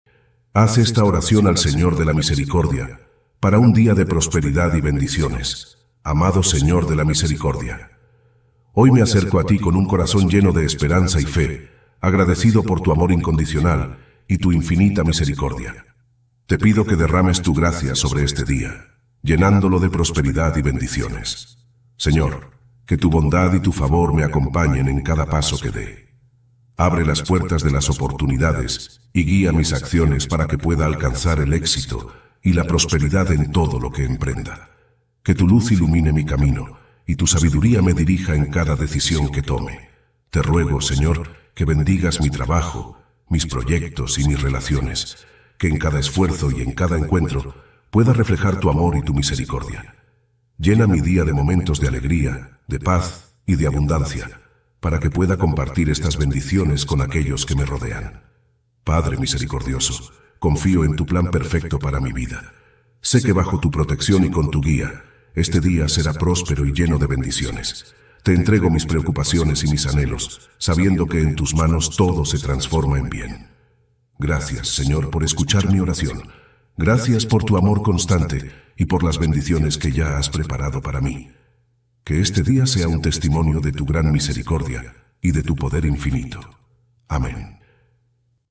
ORACION DE HOY - Haz esta Oración al Señor de la MISERICORDIA
Oración al Señor de la Misericordia para un Día de Prosperidad y Bendiciones

Amado Señor de la Misericordia,

Hoy me acerco a Ti con un corazón lleno de esperanza y fe, agradecido por Tu amor incondicional y Tu infinita misericordia. Te pido que derrames Tu gracia sobre este día, llenándolo de prosperidad y bendiciones.

Señor, que Tu bondad y Tu favor me acompañen en cada paso que dé. Abre las puertas de las oportunidades y guía mis acciones para que pueda alcanzar el éxito y la prosperidad en todo lo que emprenda. Que Tu luz ilumine mi camino, y Tu sabiduría me dirija en cada decisión que tome.

Te ruego, Señor, que bendigas mi trabajo, mis proyectos y mis relaciones. Que en cada esfuerzo y en cada encuentro, pueda reflejar Tu amor y Tu misericordia. Llena mi día de momentos de alegría, de paz y de abundancia, para que pueda compartir estas bendiciones con aquellos que me rodean.

Padre misericordioso, confío en Tu plan perfecto para mi vida. Sé que bajo Tu protección y con Tu guía, este día será próspero y lleno de bendiciones. Te entrego mis preocupaciones y mis anhelos, sabiendo que en Tus manos todo se transforma en bien.

Gracias, Señor, por escuchar mi oración. Gracias por Tu amor constante y por las bendiciones que ya has preparado para mí. Que este día sea un testimonio de Tu gran misericordia y de Tu poder infinito.

Amén.